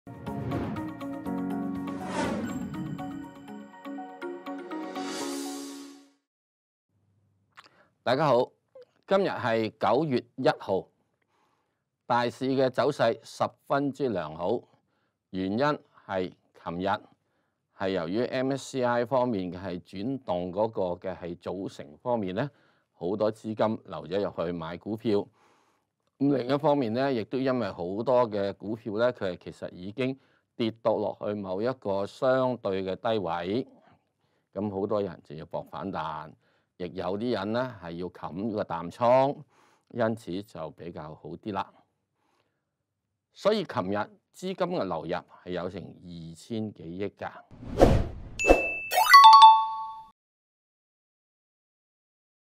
【石Sir午市閒談】今日推介恒地｜呷哺呷哺、首鋼資源、中交建、招行、京東健康最新部署(2021年9月01日)
恒生指數中午收報26038點，升159點。期指應如何部署？《香港經濟日報》專欄作者石鏡泉（石Sir）推介恒地（00012），另亦會分析呷哺呷哺（00520）、首鋼資源（00639）、中交建（01800）、招行（03968）、京東健康（06618）最新部署，詳情待石Sir為你分析。